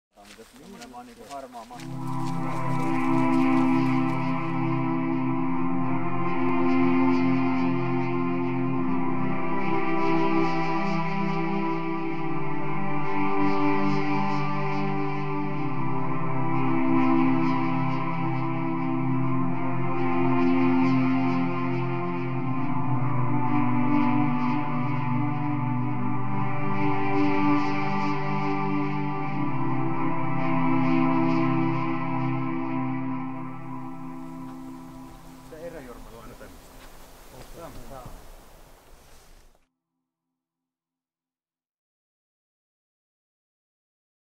Behind the scenes - Korpi ForRest Kurjenrahka National park
The first steps of making our promotional videos together with Aleksi Saariranta.